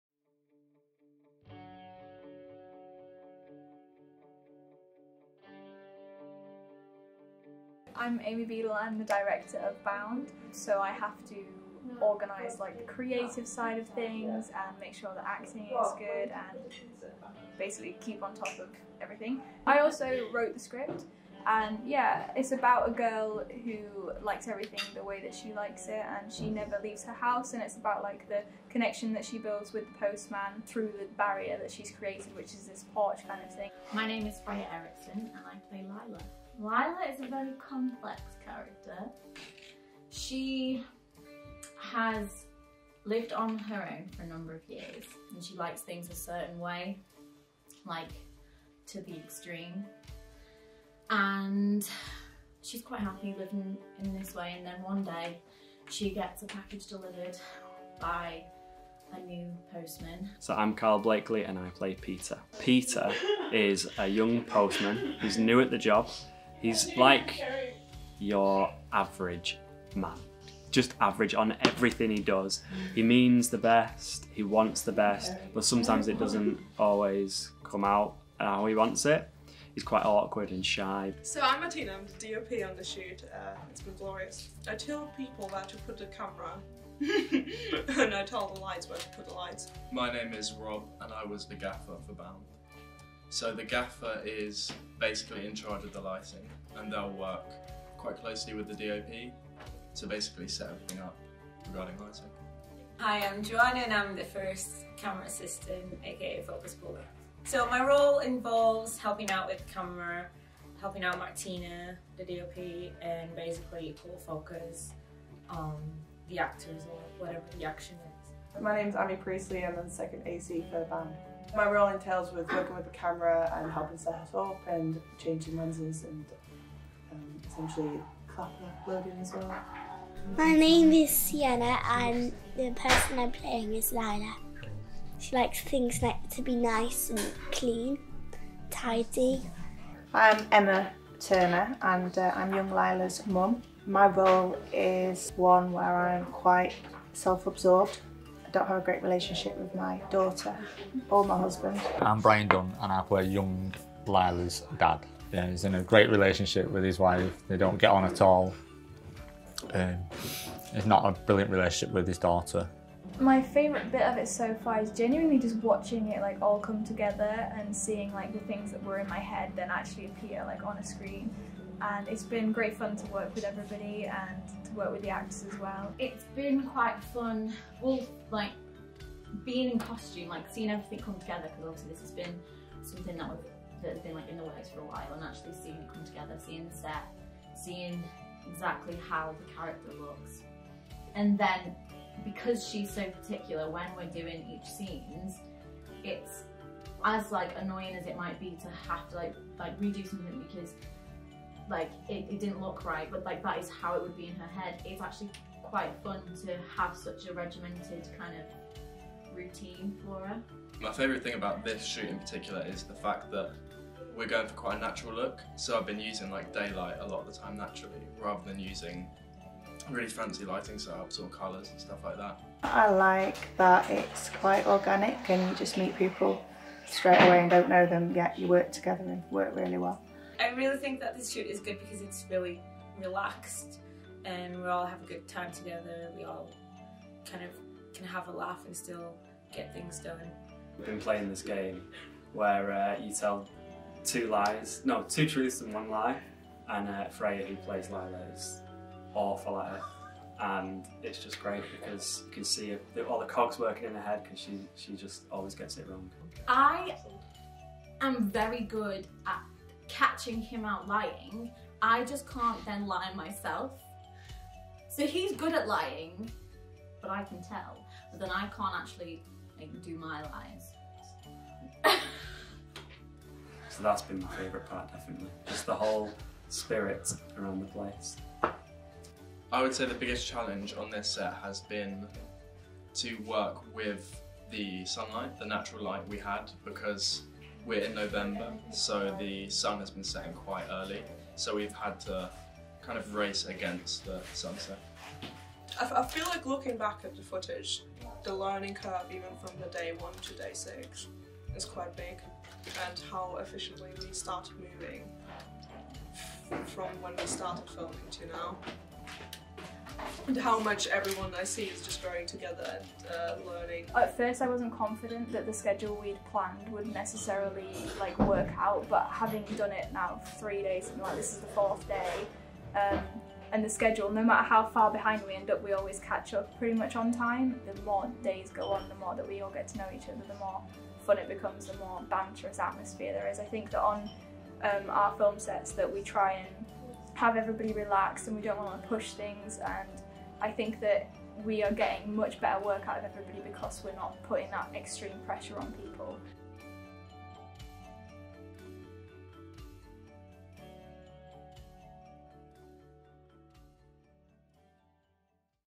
Bound | Behind The Scenes
We're excited to share with you the making of Bound!

Thank you to our great cast and crew!

'Bound' follows Lila, an agoraphobic woman, who builds an unconventional friendship with the new postman delivering in her area.

Amy Beedle and Martina Majcen :)